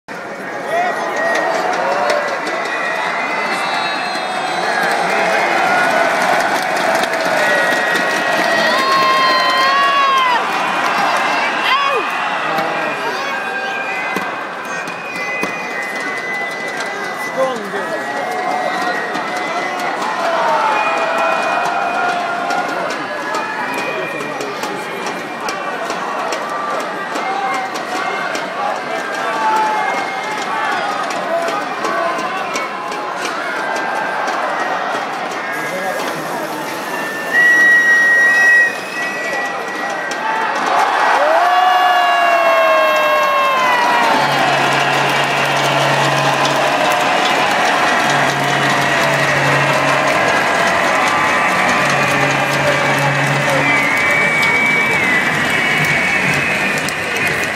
KHL All Star Game 2012 Кольцов забивает шайбу в ворота Фёдорова
Матч Звёзд 2012 - KHL All Star Game 2012 Кольцов забивает шайбу в ворота Фёдорова